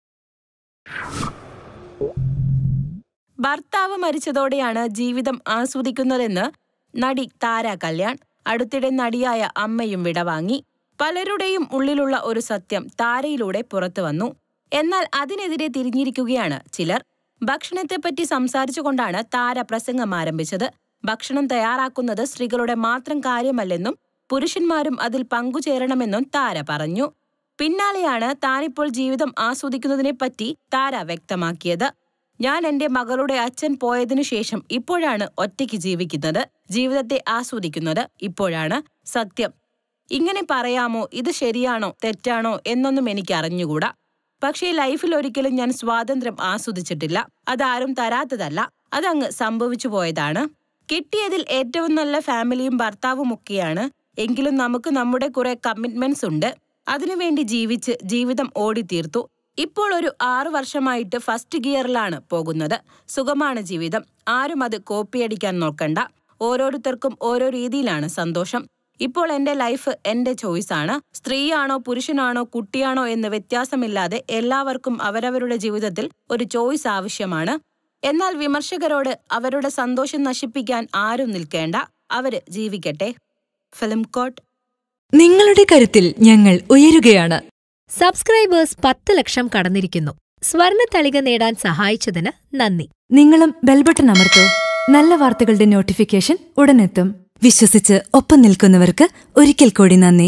ഭർത്താവ് മരിച്ചതോടെയാണ് ജീവിതം ആസ്വദിക്കുന്നതെന്ന് നടി താര കല്യാൺ.അടുത്തിടെ നടിയായ അമ്മയും വിടവാങ്ങി
Watch ഭർത്താവ് മരിച്ചതോടെയാണ് ജീവിതം ആസ്വദിക്കുന്നതെന്ന് നടി താര കല്യാൺ.. അടുത്തിടെ നടിയായ അമ്മയും വിടവാങ്ങി